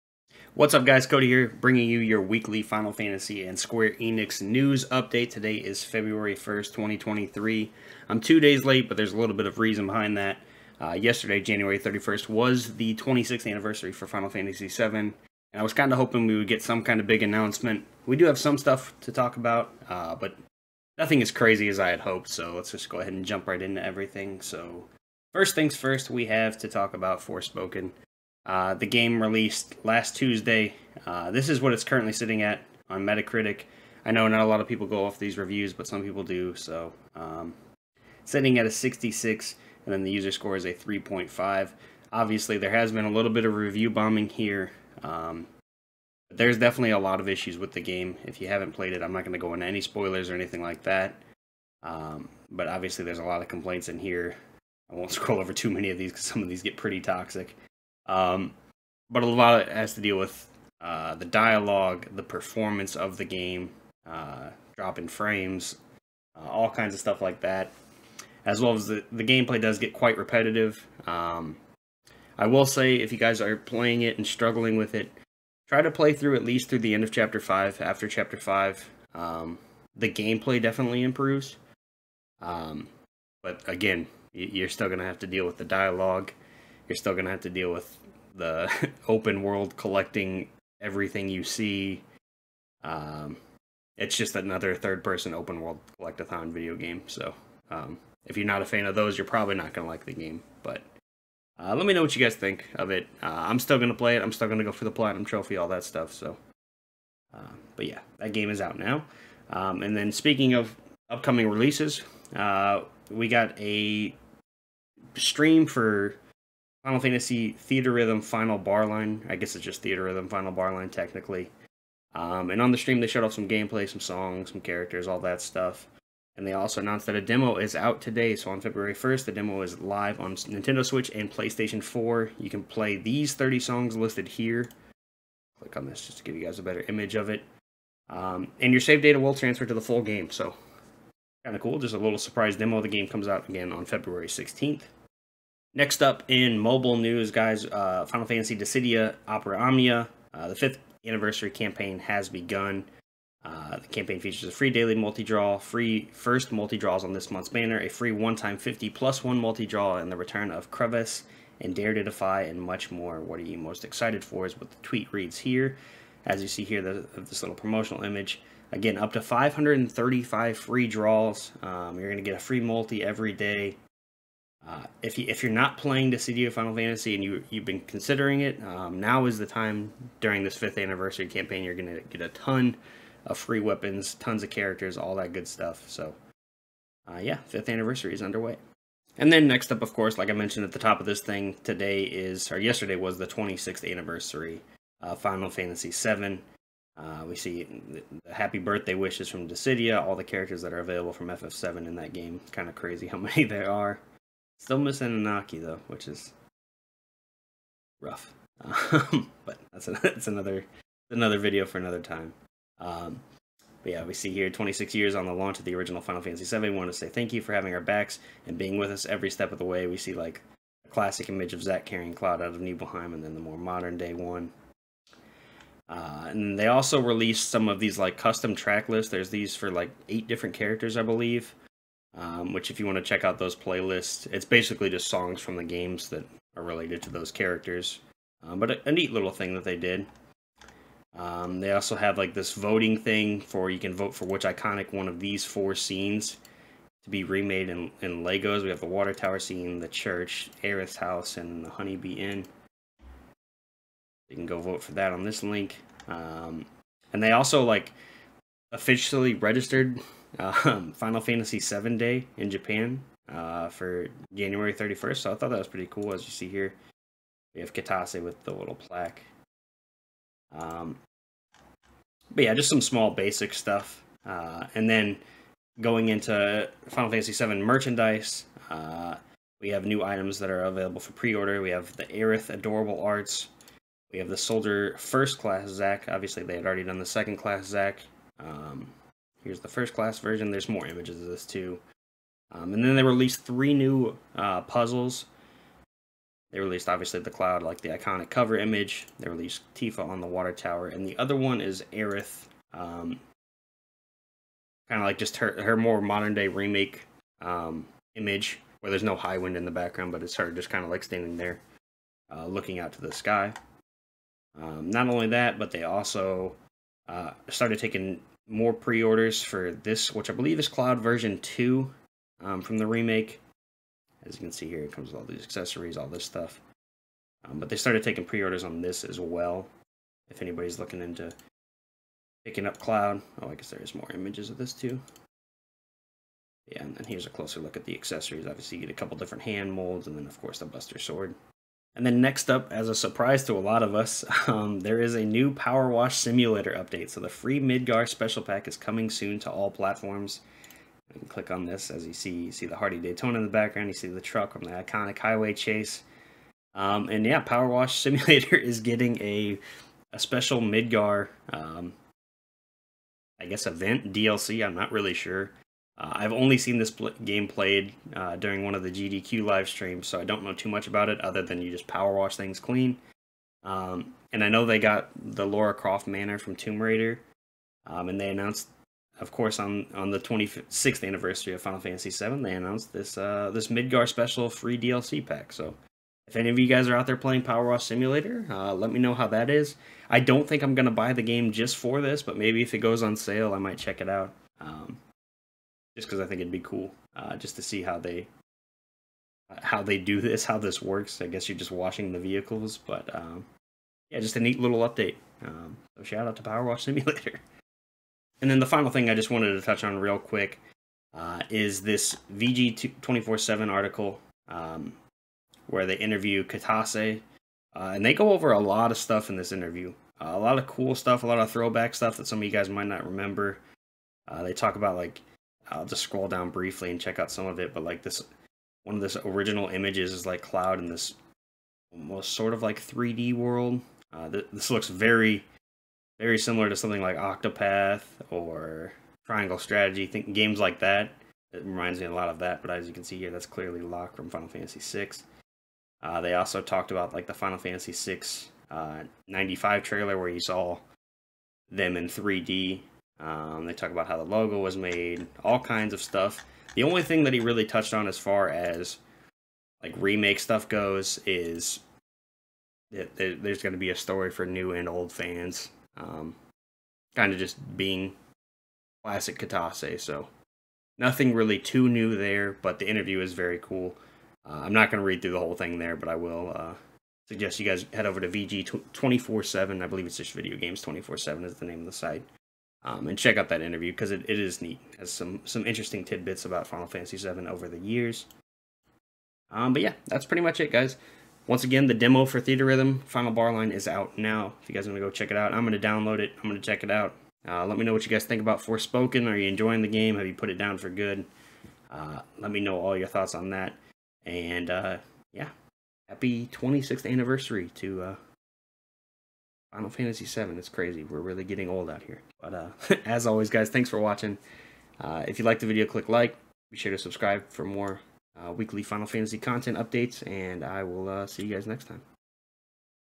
FINAL FANTASY VII TURNS 26, FORSPOKEN REVIEWS, THEATRHYTHM FINAL BARLINE DEMO
In todays video I am going over all of the News that happened this past week in regards to Final Fantasy and other Square Enix related products.